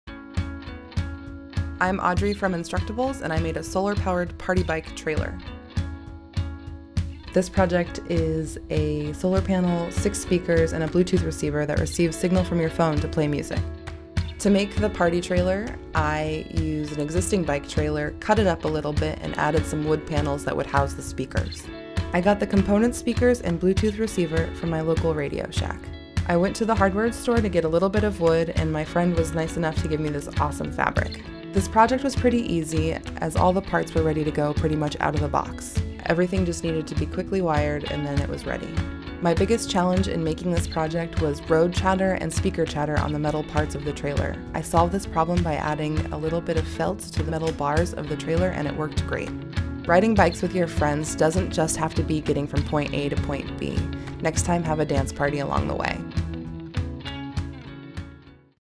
Solar Powered Disco Bike Trailer
Building this Solar Power Bike Trailer was probably the most fun I've ever had during a build. (Really anytime I get to use a cut-off wheel on an angle grinder, I'm pleased.) Furthermore, I'm excited for the years of fun I will have with this trailer, towing a portable dance party behind my bike! My friends and I will have the best pedal-powered dance party in the streets! Be sure to join us if you ever see us riding around :)